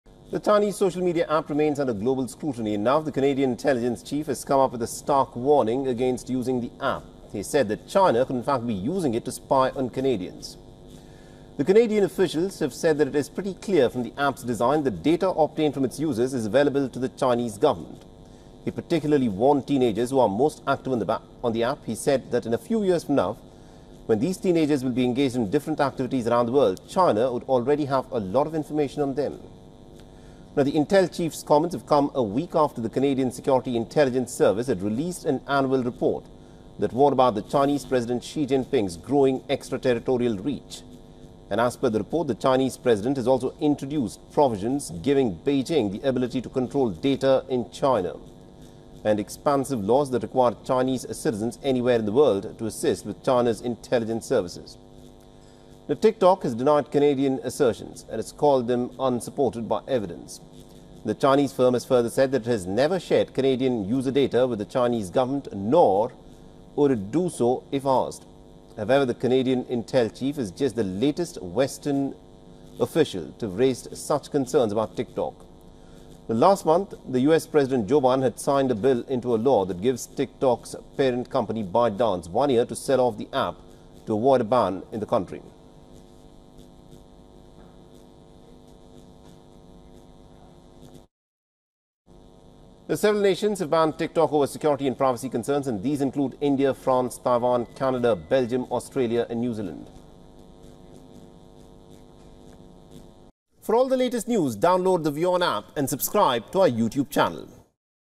TikTok: Firm never shared user data with Chinese government | World News | WION
The Chinese social media app remains under Global scrutiny. The Canadian intelligence Chief has come up with a stark warning against using the app. He said that China could in fact be using it to spy on Canadians. Watch in for more details!